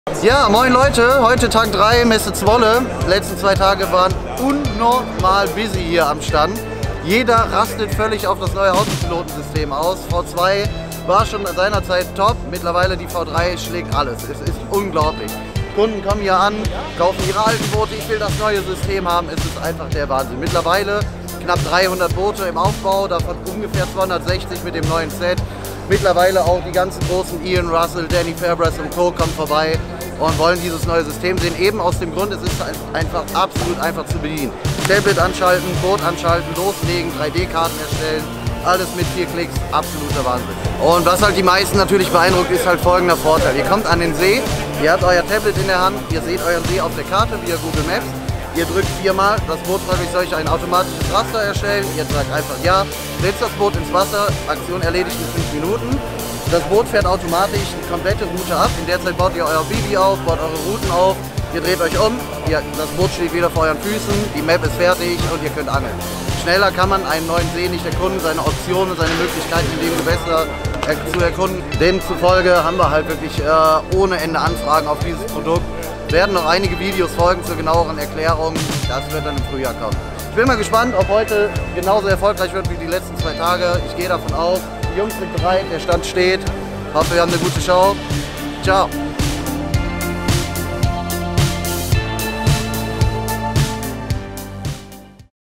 Carplounge @CarpZwolle -Daniel Radmacher zum RT4 Raymarine-GPS-Autopilot Mapping Set
Carplounge @ Carp Zwolle 2018 - Aftersales Manager Daniel Radmacher über das Raymarine-GPS-Autopilot Set mit 3D Mapping.